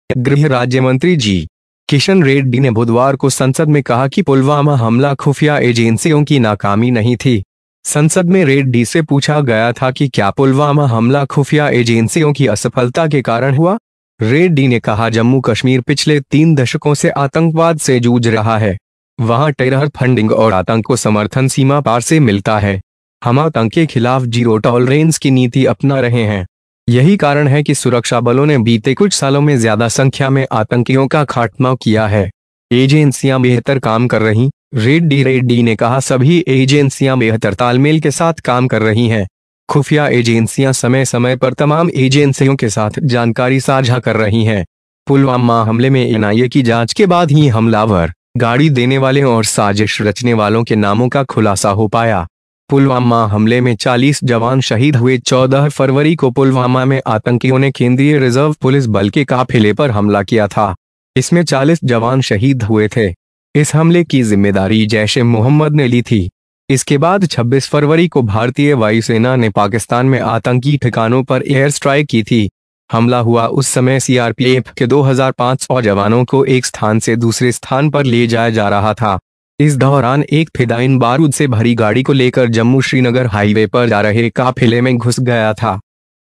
पुलवामा हमले पर सरकार का संसद में जवाब
Minister of State for Home G.Kishan Reddy said in Parliament on Wednesday that the Pulwama attack was not a failure of intelligence agencies. In the Parliament, Reddy was asked whether the Pulwama attack was due to the failure of intelligence agencies? Reddy said: Jammu and Kashmir has been fighting terrorism for the past three decades. Terror funding and terror support are available across the border. We are pursuing Zero Tolerance policy against terror. This is the reason that the security forces have eliminated a large number of terrorists in the last few years. The Agencies were working better: Reddy Reddy said - All the agencies are working with better coordination. Intelligence agencies are sharing information from time to time with all agencies. The names of the attackers, cartlers and conspirators came to light only after the investigation of the NIA in the Pulwama attack. 40 people were killed in the Pulwama attack On February 14, the militants attacked the convoy of the Central Reserve Police Force in Pulwama. There were 40 soldiers in this martyr The responsibility was taken by Jaish-e-Mohammed. After this, on February 26, the Indian Air Force had made an air strike on the terror bases in Pakistan. At that time, 2500 CRPF personnel were being taken from one place to another. During this, a fidayeen had entered the convoy going on the Jammu-Srinagar highway with a loaded vehicle.